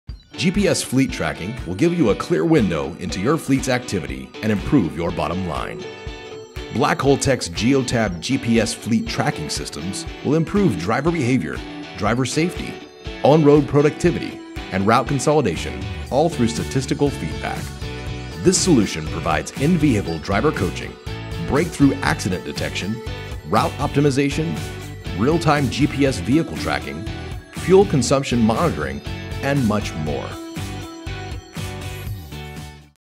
Blackhole Tech GPS Tracking Systems
Our team at Blackhole Tech installs and services Geotab GPS fleet tracking systems that benefit your bottom line. Receive statistical feedback on your fleet activity, and improve driver behavior, driver safety, on road productivity and route consolidation. This system also offers driver coaching and fuel monitoring, so you get the most bang for your buck.